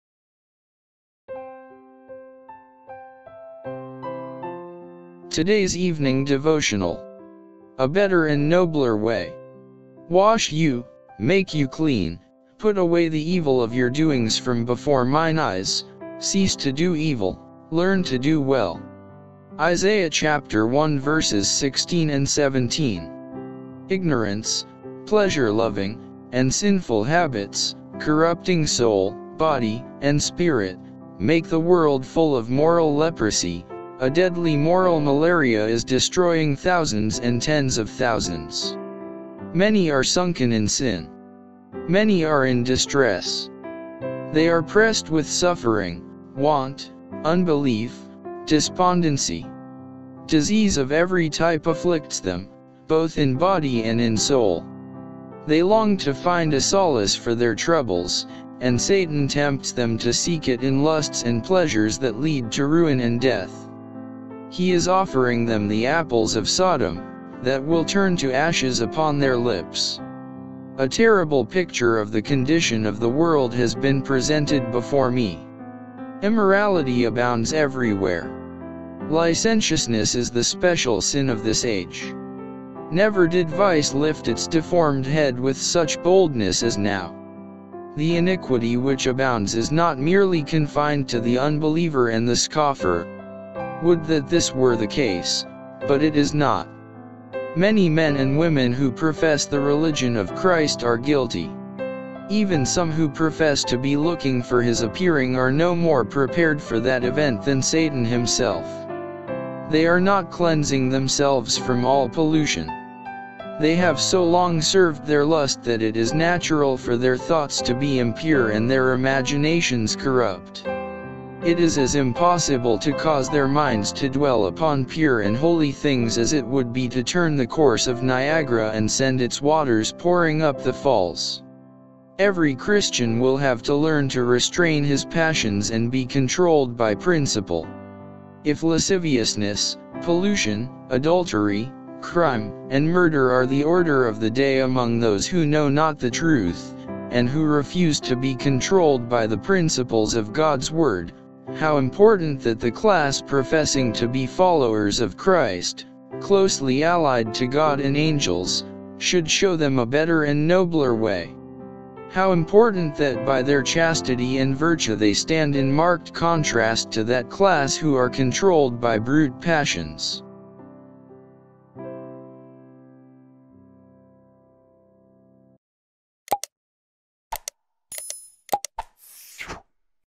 A Better and Nobler Way - Today's Evening Devotional 01-17-21
Daily Devotion

A Better and Nobler Way

Wash you, make you clean; put away the evil of your doings from before mine eyes; cease to do evil; learn to do well. Isaiah 1:16, 17

Ignorance, pleasure loving, and sinful habits, corrupting soul, body, and spirit, make the world full of moral leprosy; a deadly moral malariaac is destroying thousands and tens of thousands.

Many are sunken in sin. Many are in distress. They are pressed with suffering, want, unbelief, despondency. Disease of every type afflicts them, both in body and in soul. They long to find a solace for their troubles, and Satan tempts them to seek it in lusts and pleasures that lead to ruin and death. He is offering them the apples of Sodom, that will turn to ashes upon their lips.

A terrible picture of the condition of the world has been presented before me. Immorality abounds everywhere. Licentiousness is the special sin of this age. Never did vice lift its deformed head with such boldness as now. The iniquity which abounds is not merely confined to the unbeliever and the scoffer. Would that this were the case, but it is not. Many men and women who profess the religion of Christ are guilty. Even some who profess to be looking for His appearing are no more prepared for that event than Satan himself. They are not cleansing themselves from all pollution. They have so long served their lust that it is natural for their thoughts to be impure and their imaginations corrupt. It is as impossible to cause their minds to dwell upon pure and holy things as it would be to turn the course of Niagara and send its waters pouring up the falls. Every Christian will have to learn to restrain his passions and be controlled by principle.

If lasciviousness, pollution, adultery, crime, and murder are the order of the day among those who know not the truth, and who refuse to be controlled by the principles of God's word, how important that the class professing to be followers of Christ, closely allied to God and angels, should show them a better and nobler way! How important that by their chastity and virtue they stand in marked contrast to that class who are controlled by brute passions!